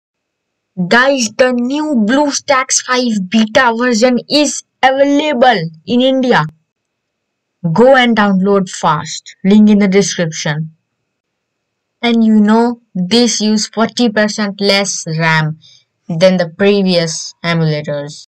Bluestacks 5 !
guys the new bluestack 5 is out in india and this emulator uses 40% less ram and claim that it can run on low end PCs so go and download it now!